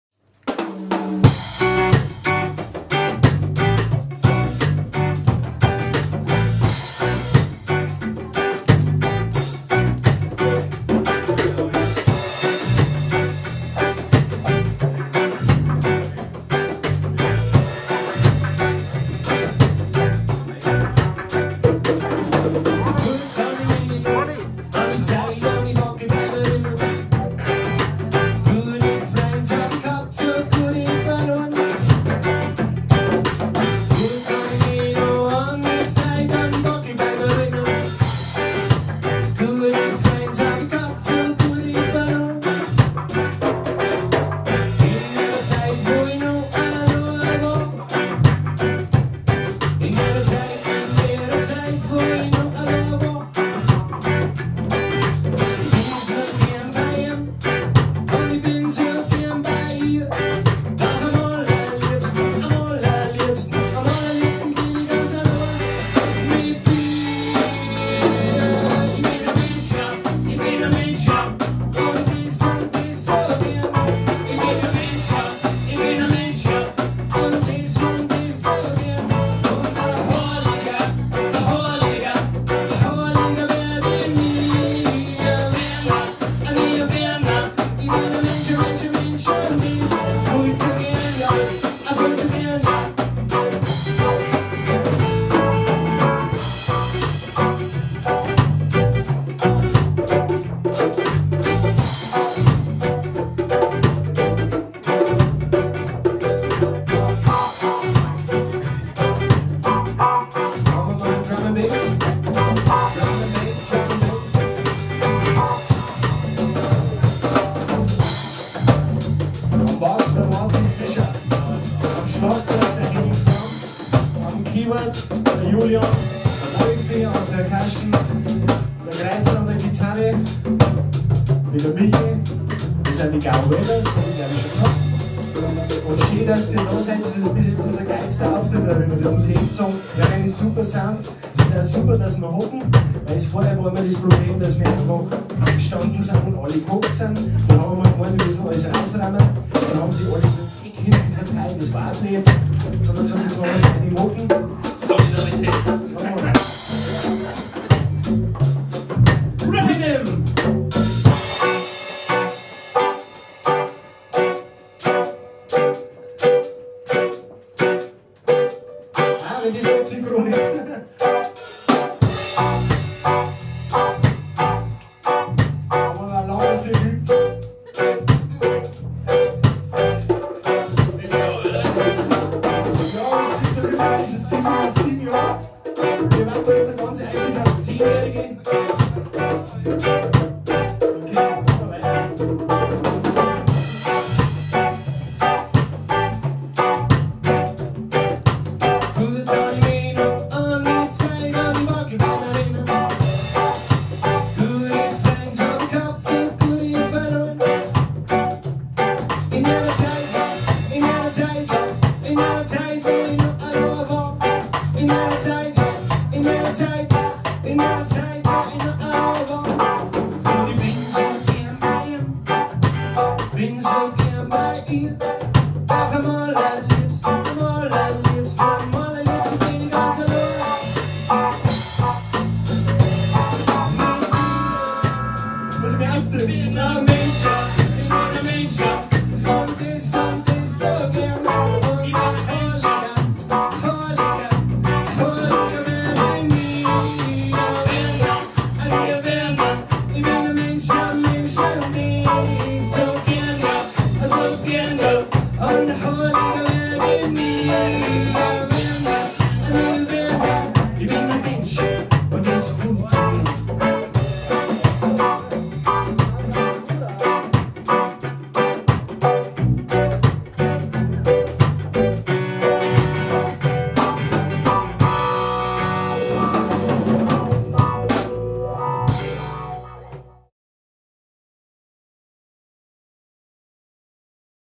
The Gauwailers live at Kneipenbühne, May 16, 2009
Bissiger, erdverbundener und traditionsbewusster Reggae mit sozialkritischen Texten.
Wie muss man sich das vorstellen? Und vor allem: Funktioniert das heute überhaupt? Miche Süllner, Frontmann und Mastermind der Formation "The Gauwailers", grinst seine vier Bandmitglieder an. Die grinsen zurück und haben dadurch die Frage eigentlich schon mit einem eindeutigen "Ja" beantwortet. Aus einer spontanen Idee wurde Realität.